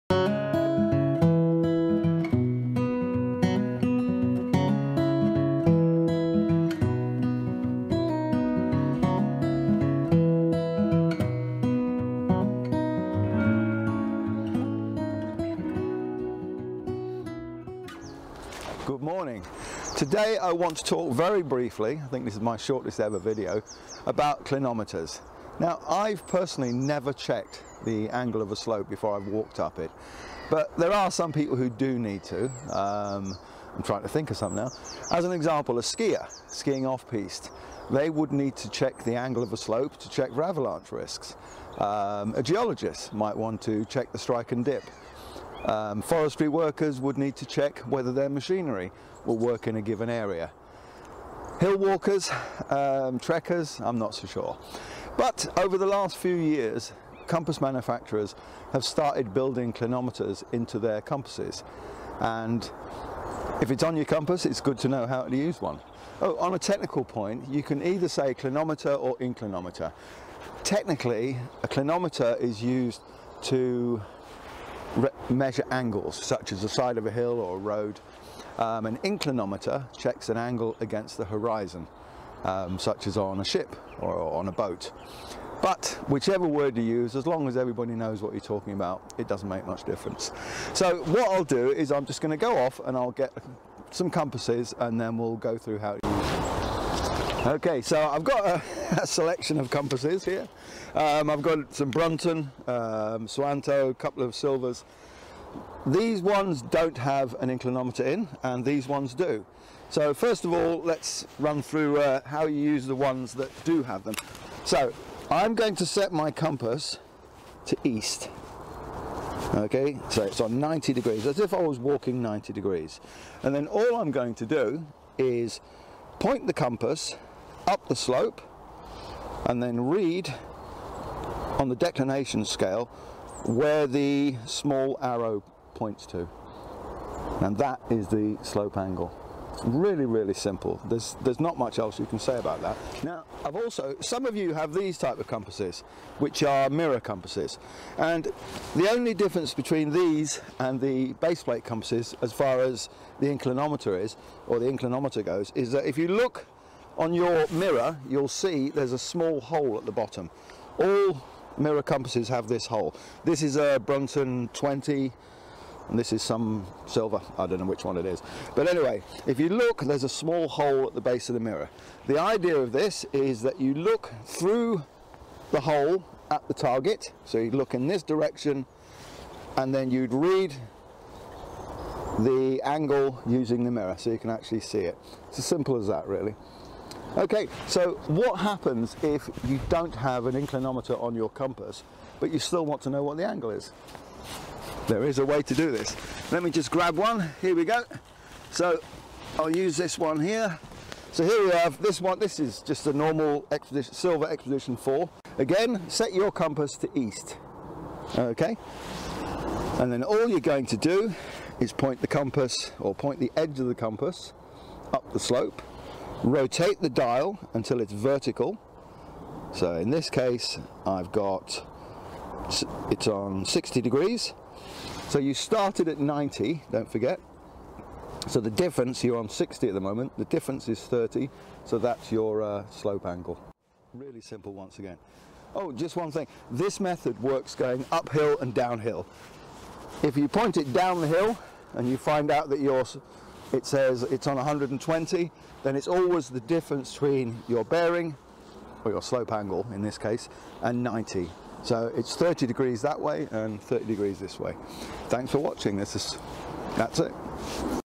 Slope angle with or without a clinometer
You don’t need a clinometer to check the angle of a slope.  In this video I have demonstrated how to use a clinometer and also how to get the angle if you don’t have one.